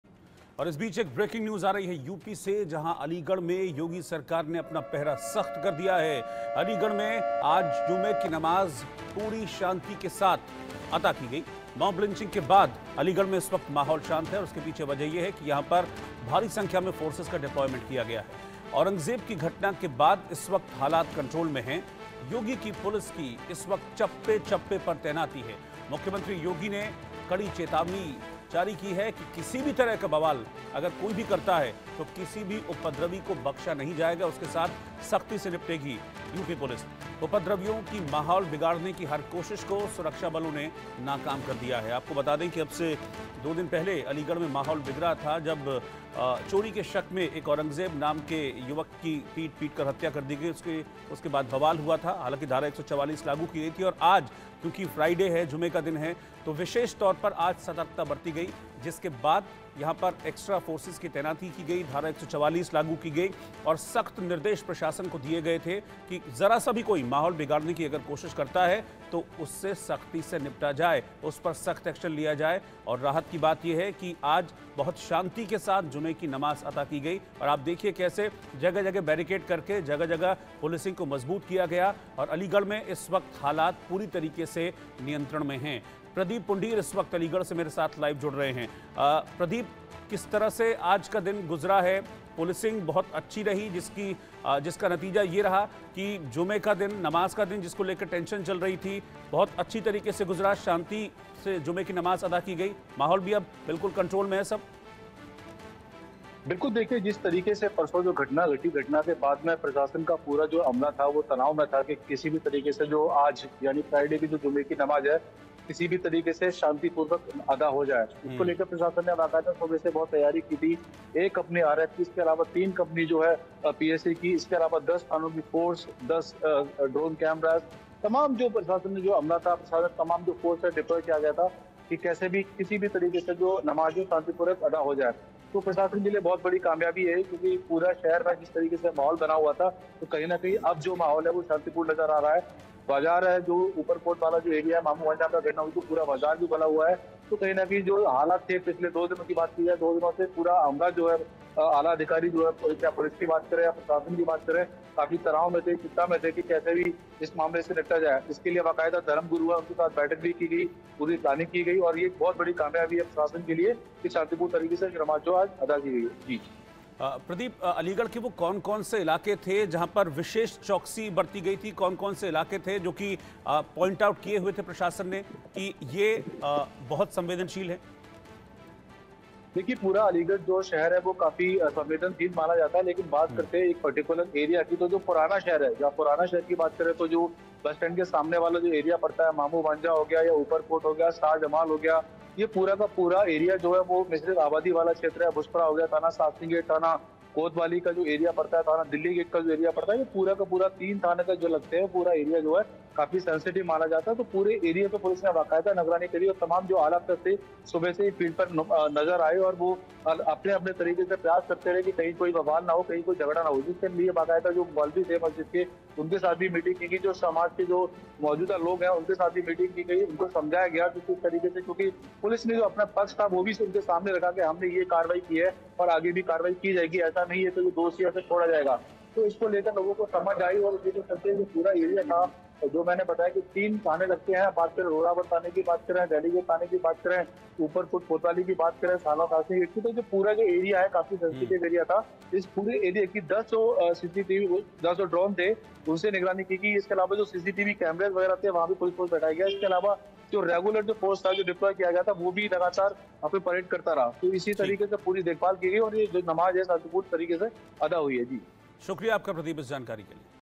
CM Yogi Action against Aligarh Mob Lynching Case: यूपी पुलिस की निगरानी में पड़ी गई नमाज और फिर....
Aligarh Mob Lynching News : अलीगढ़ में पथराव के बाद जबरदस्त तनाव का माहौल बन गया। इस पथराव के बीच दो समुदाय के लोग आमने सामने आ गए। अलीगढ़ के मामू भांजे इलाके में दूसरे समुदाय के युवक की पीट-पीटकर हत्या करने का मामला सामने आया, जिसके बाद दो पक्षों के बीच पथराव शुरू हो गया। मौके पर भारी पुलिस बल और वरिष्ठ अधिकारी तैनात हैं। ये घटना थाना कोतवाली इलाके के अब्दुल करीम चौराहा की है।

Aligarh Mob Lynching News: After stone pelting, an atmosphere of tremendous tension was created in Aligarh. Amidst this stone pelting, people of two communities came face to face. A case of lynching of a youth from another community came to light in Mamu Bhanje area of ??Aligarh, after which stone pelting started between the two parties. Heavy police force and senior officers are deployed at the spot. This incident took place at Abdul Karim intersection of Kotwali police station area.